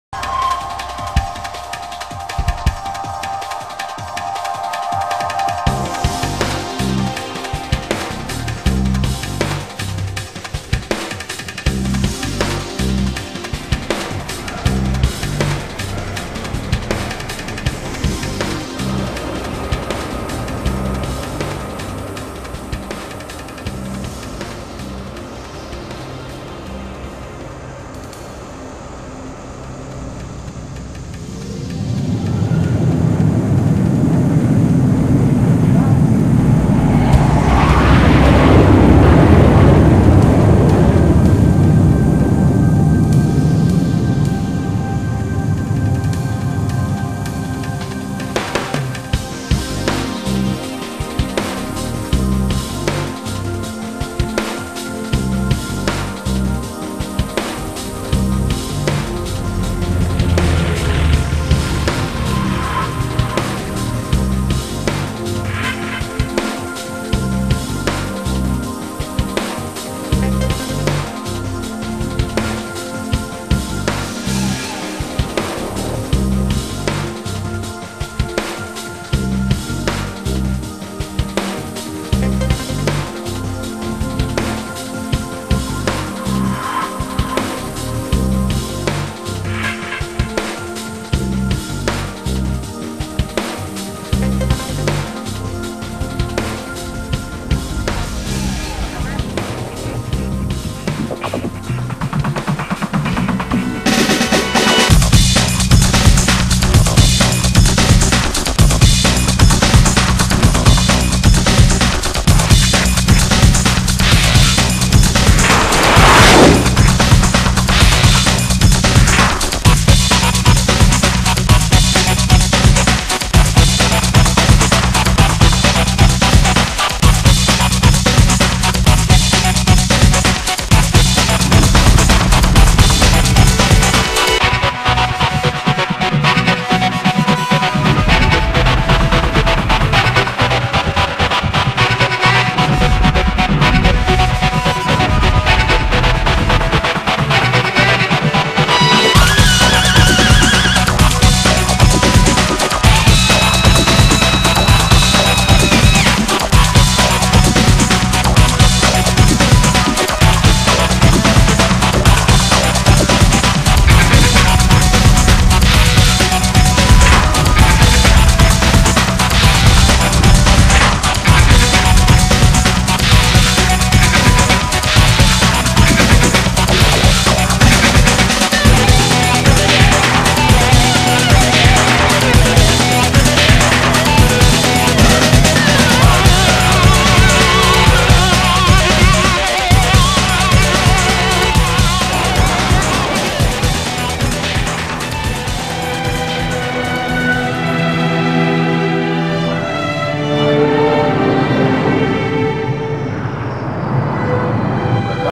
Lockheed Martin MRE unmanned aircraft
Lockheed Martin video animation from 2001 showing its concept for a US Navy carrier-based Multi-Role Endurance (MRE) umanned aircraft.